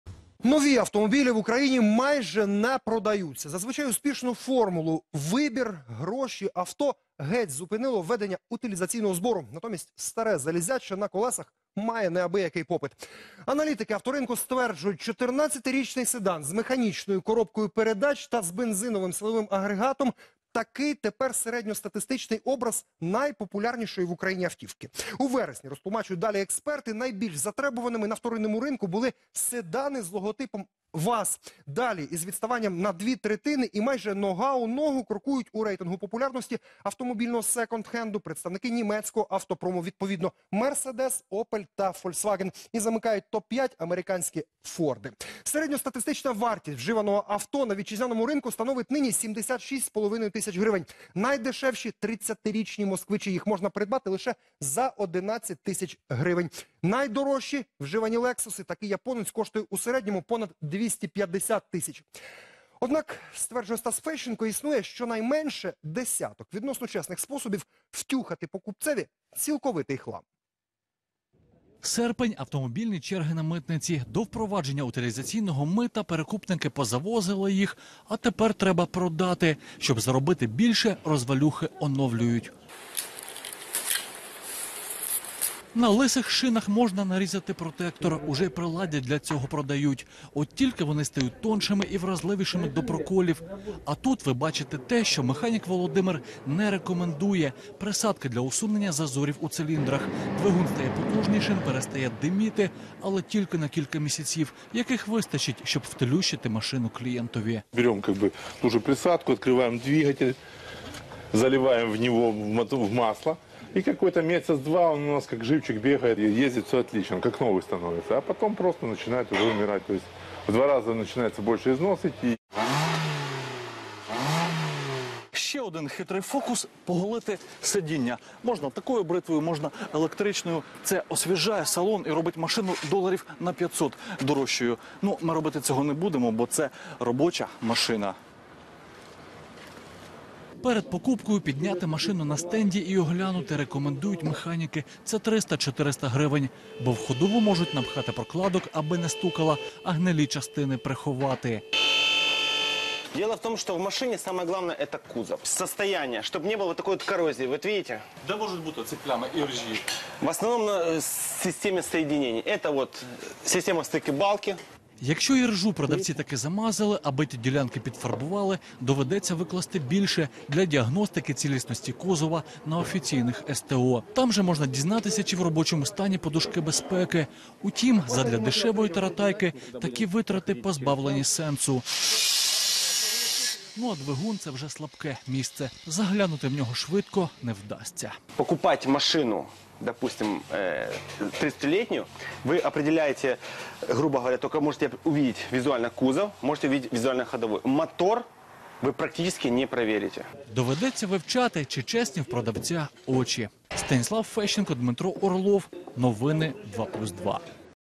Новини українського авторинку
Нові автомобілі в Україні майже не продаються.  Зазвичай успішну формулу «вибір-гроші-авто» геть зупинило введення утилізаційного збору. Натомість старе залізяччя на колесах має неабиякий попит.
Аналітики авто ринку стверджують: 14-річний седан з механічною коробкою передач  та з бензиновим силовим агрегатом  - такий тепер середньостатистичний образ найпопулярнішої в Україні автівки. У вересні, розтлумачують далі експерти, найбільш затребуваними на вторинному ринку були ВАЗ, далі із відставанням у 2/3 і майже нога у ногу крокують  у рейтингу популярності автомобільного «секонд-хенду» представники німецького автопрому -- відповідно Мереседес, Опель та Фольксваген. Замикають Топ-5 американські Форди.
Статистична вартість вживаного авто на вітчизняному ринку становить нині 76,5 тисяч гривень. Найдешевші 30-річні Москвичі, їх можна придбати лише за 11 тисяч гривень. Найдорожчі -- вживані Лексуси. Такі японці у середньому коштують понад 250 тисяч.
Однак, існує щонайменше десяток чесних способів втюхати покупцеві цілковитий хлам.